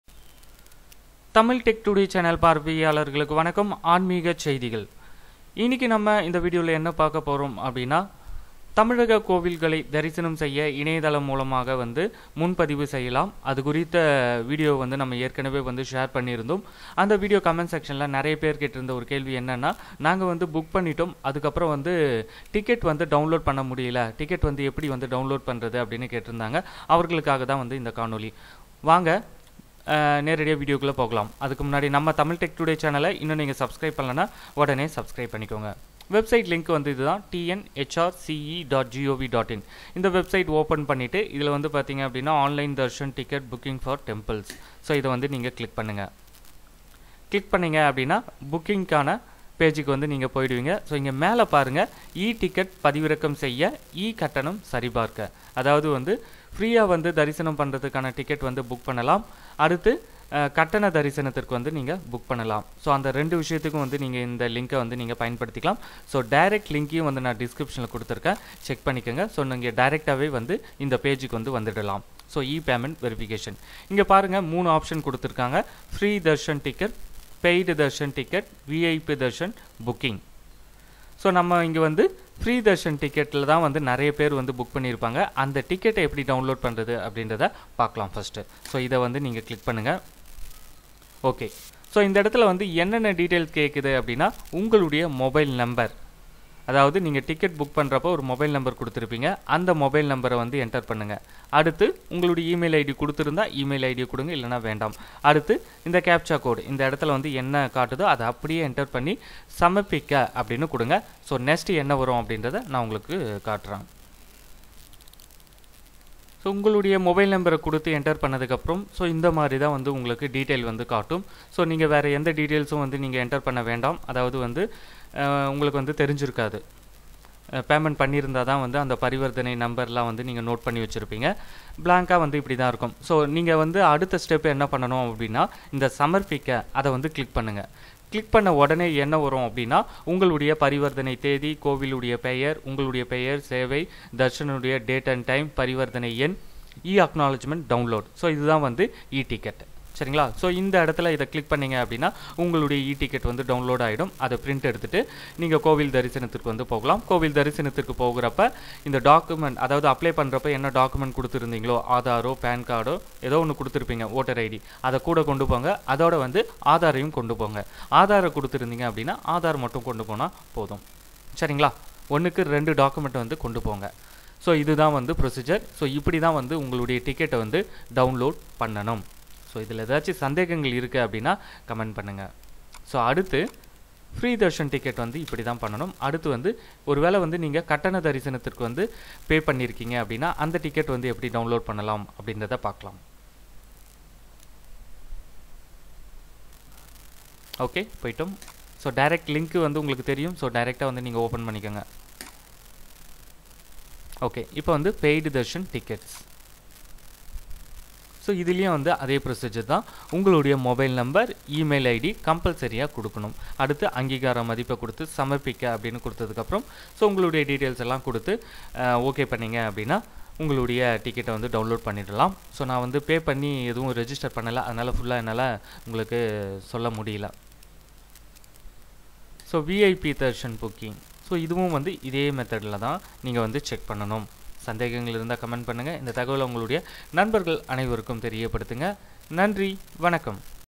2020 தமிழக கோவில்கள்|ஆன்லைன் தரிசன டிக்கெட் Download செய்வது எப்படி? tnhrce.gov.in
tnhrce 2020|Online Darshan Ticket Booking Download Online in Tamil|தமிழக கோயில் தரிசனம் முன்பதிவு
டிக்கெட் Download செய்வது எப்படி?

Online Darshan Ticket Booking|TNHRCE 2020|e-Payment Verification